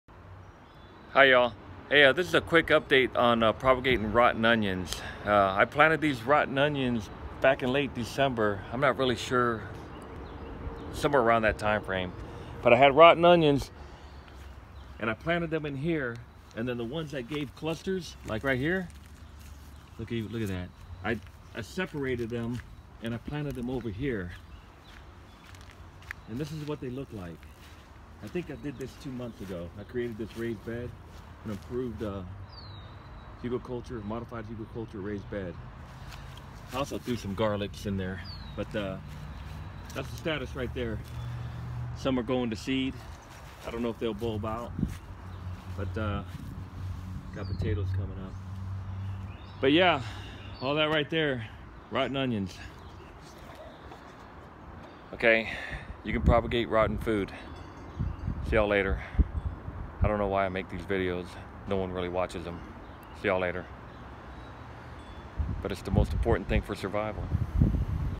Propagating rotten onions update
It is going on three months since the rotten onion shoots were separated from the main rotten onions. The growing process is looking promising. Here is a link to one of the earlier videos.

Propagating Rotten Onion Seeds
Propagating and collecting rotten onion seeds.